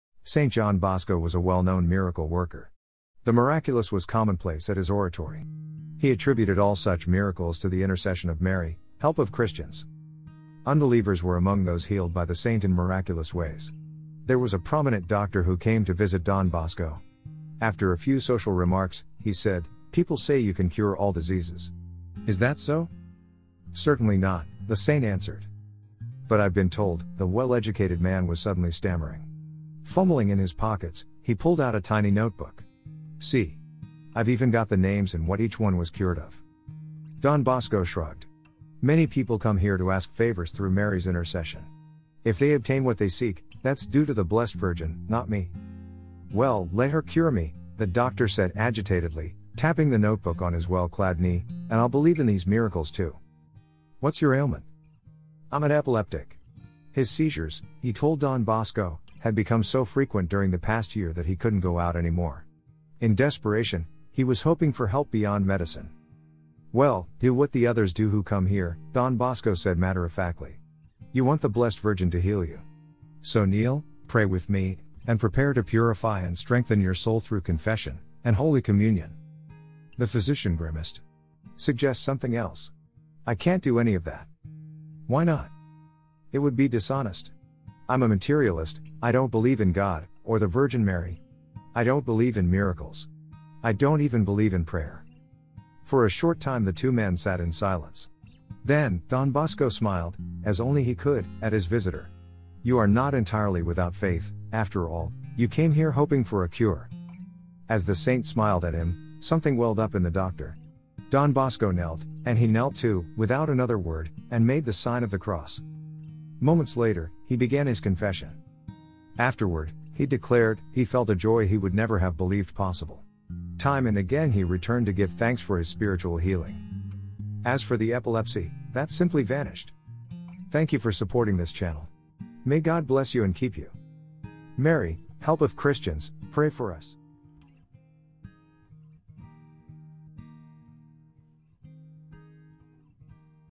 Don Bosco, A Healing Miracle and Mary, Help of Christians
In this video we hear about Don Bosco, A healing miracle and the intercession of Mary, Help of Christians.

Pope Pius XI said, "In Don Bosco the extraordinary becomes ordinary." They were given to him in partial reward for his exceptional self-sacrifice and as a seal of divine approval of his work.

Don Bosco often told his young people that being a saint was easy. His holiness was attractive because it was rooted in charity and exceptional purity that drew people to him. Though he sometimes did extraordinary penances, he would never allow them to his boys. "Sanctity is easy!" he would say. He told his Salesians and the young people that God wants us to be happy and to rejoice in the love of Jesus. Just do your duty in school, at home, at work the best you can. Offer you life to God: the happy times and the sad or challenging things: life sends many opportunities to join in the sufferings of Jesus: bad weather, disappointments, physical illness, sorrow these will make you saints.

Many miracles were attributed to St. John Bosco and always he pointed to Mary, Help of Christians.

This page celebrates: Everything Catholic, Saints, Sacraments, Prayer and Catholic Culture.